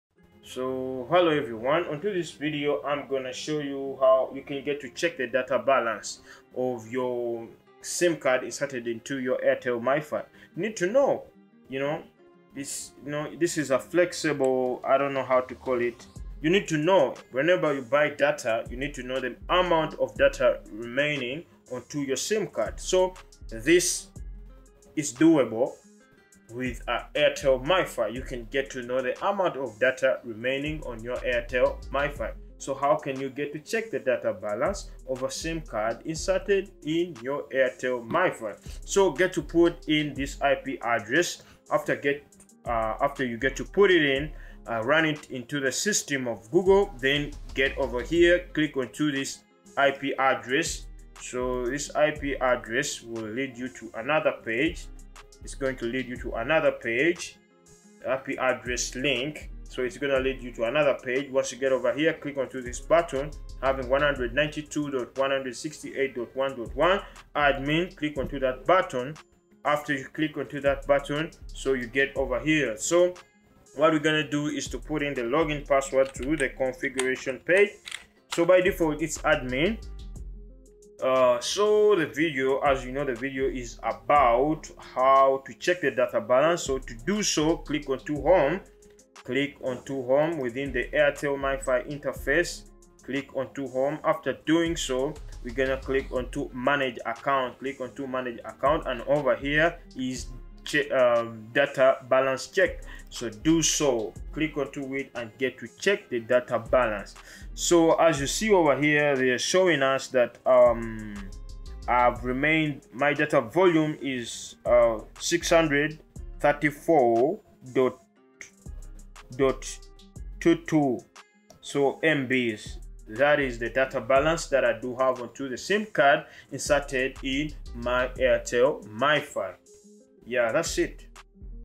HOW TO CHECK THE DATA BALANCE OF A SIM CARD IN YOUR AIRTEL MIFI/ROUTER
Welcome to Accar TechUg, In this video we are showing you how to check the data balance of a SIM Card in your Airtel mifi/Router through the website address